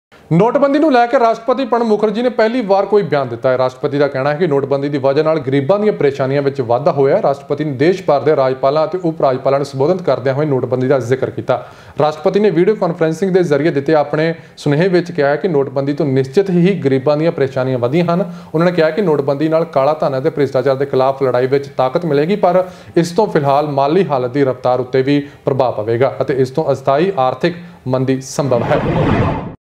Demonetisation may slow down economy President of India
ਨੋਟਬੰਦੀ ਉੱਤੇ ਪਹਿਲੀ ਵਾਰ ਬੋਲੇ ਰਾਸ਼ਟਰਪਤੀ ,ਗਰੀਬਾਂ ਦੀ ਪਰੇਸ਼ਾਨੀਆਂ ‘ਚ ਵਾਧਾ